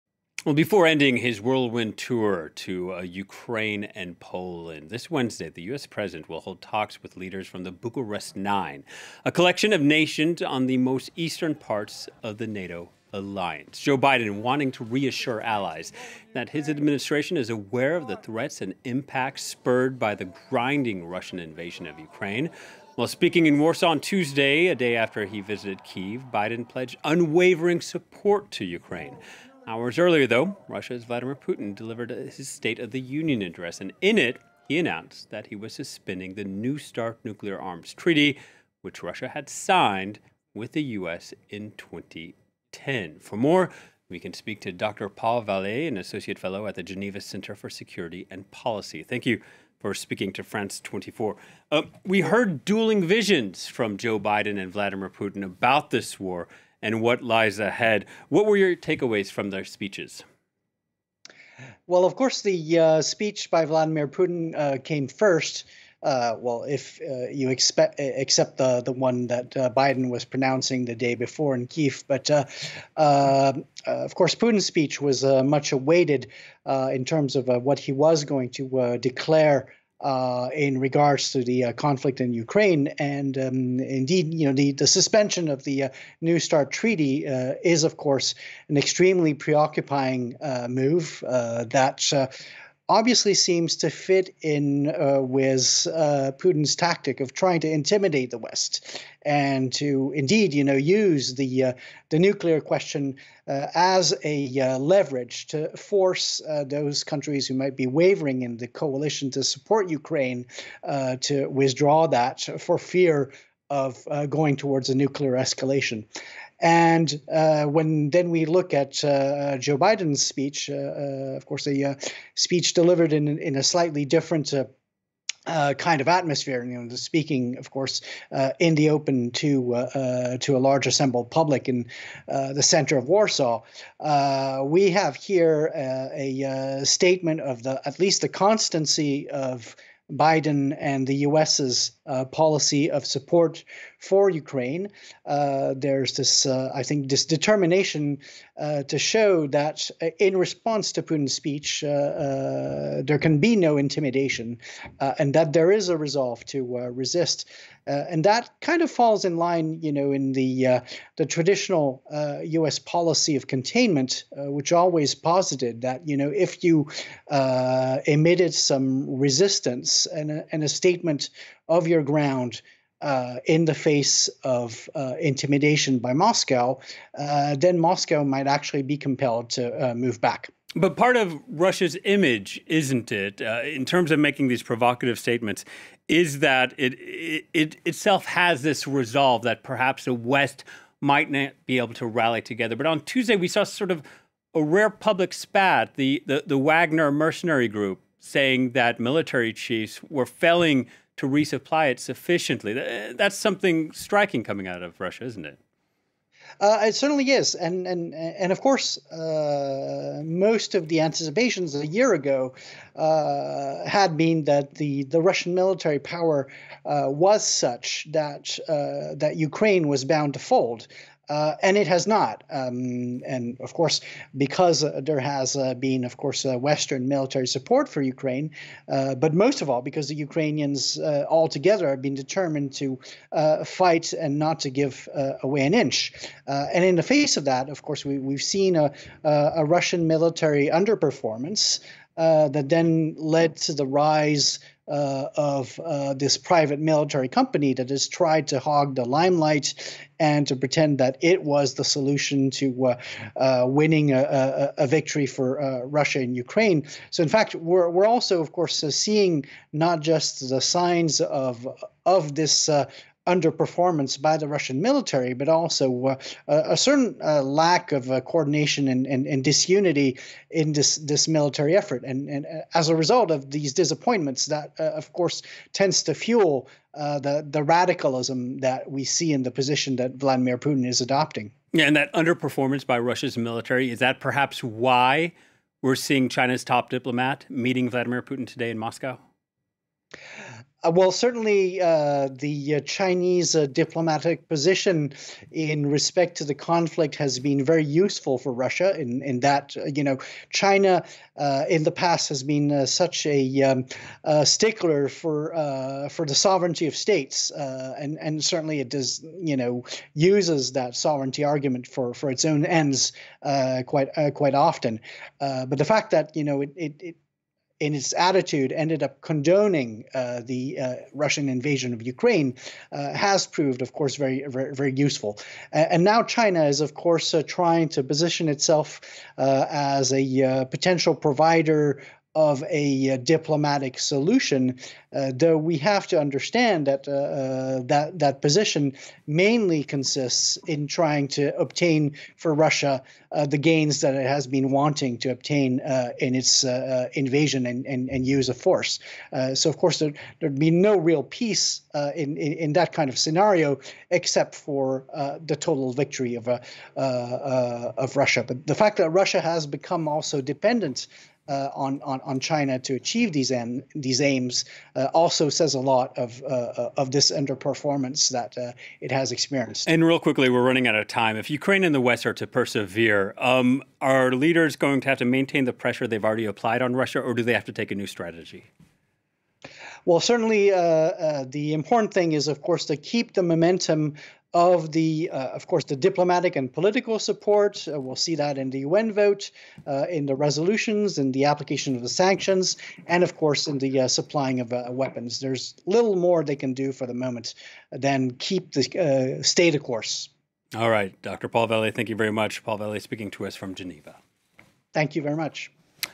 One year on, Ukraine 'determined to fight' while Russia suffers 'underperformance' and 'disunity'
For more on Russia's war on Ukraine and the dueling speeches in Warsaw and Moscow, FRANCE 24 is joined by Paul Vallet, Researcher for The Geneva Centre for Security Policy.